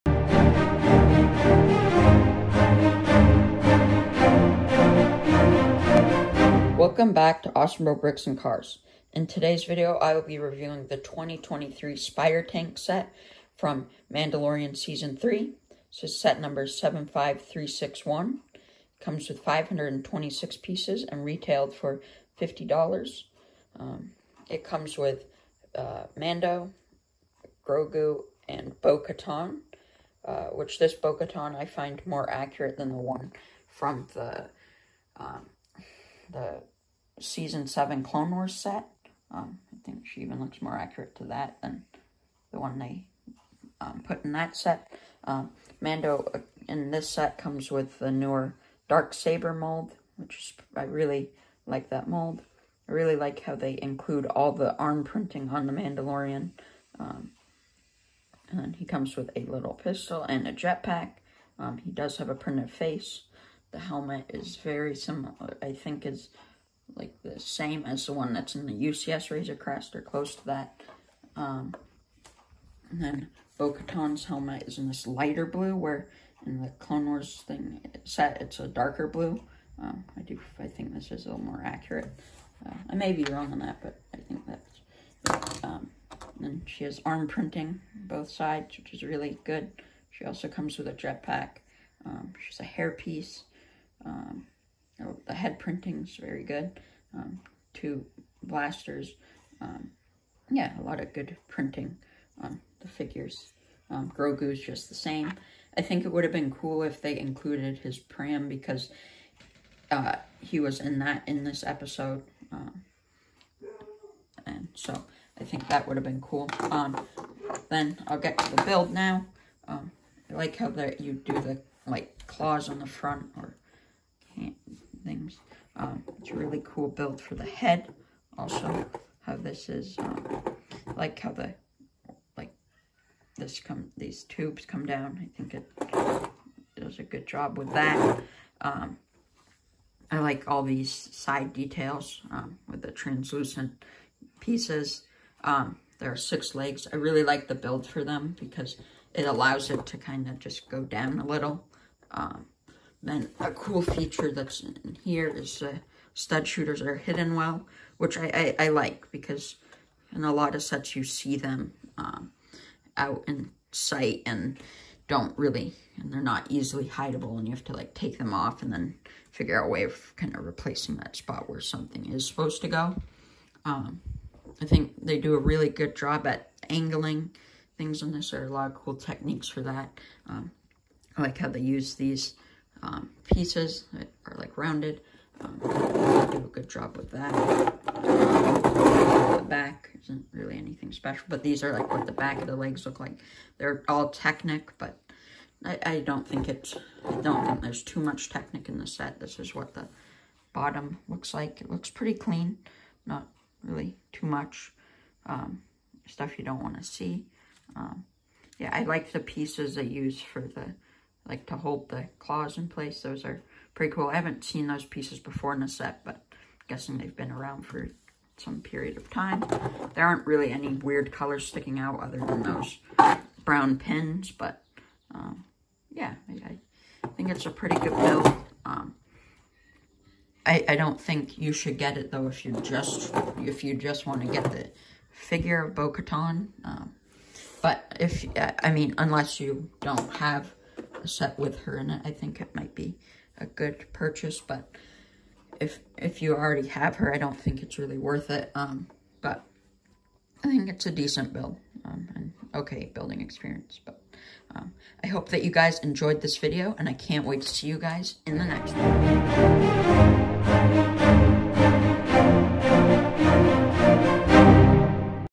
2023 LEGO Star Wars Spider Tank Review
This is the video with my thoughts on the Lego Star Wars Spider Tank Set from 2023. Make sure to put video and MOC ideas that you think I should do next.